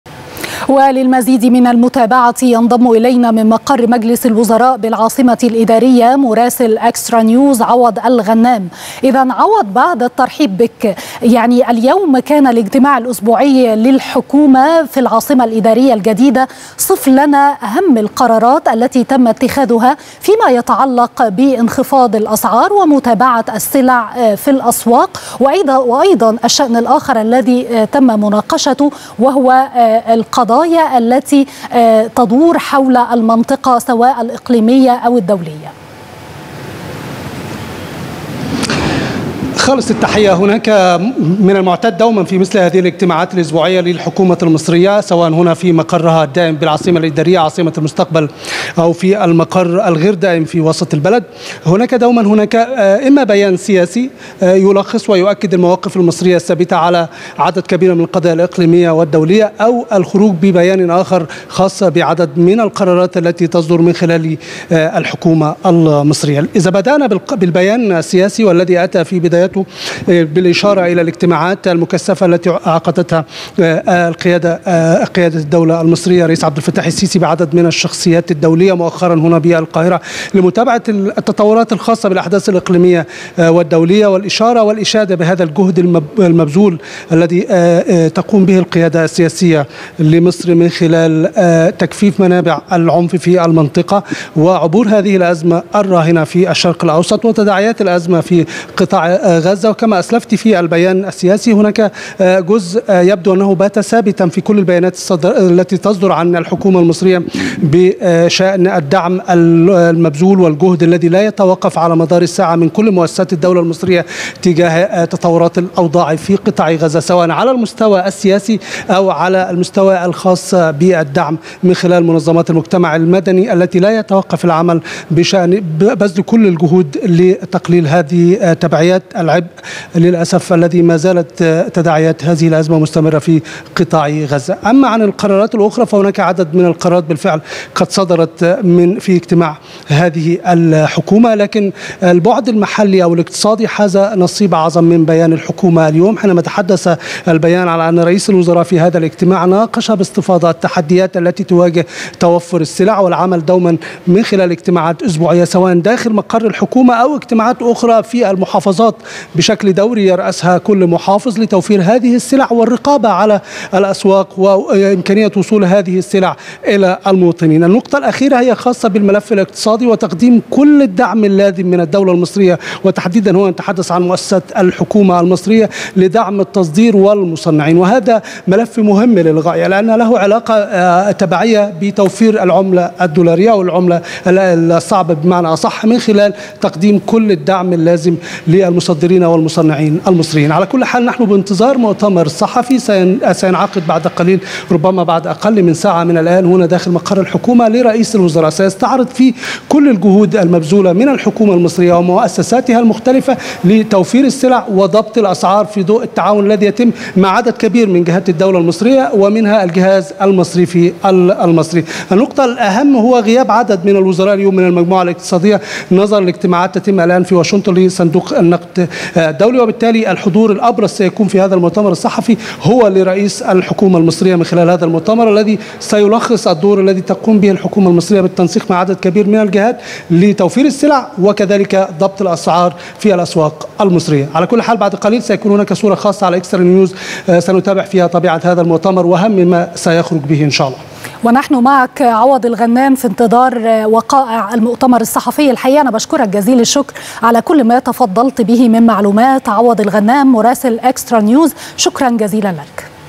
مراسلنا عوض الغنام ينقل تفاصيل مخرجات اجتماع مجلس الوزراء وأهم القرارات الخاصة بانخفاض الأسعار
جولة الـ 3 عصرا| مراسلنا عوض الغنام ينقل تفاصيل مخرجات اجتماع مجلس الوزراء وأهم القرارات الخاصة بانخفاض الأسعار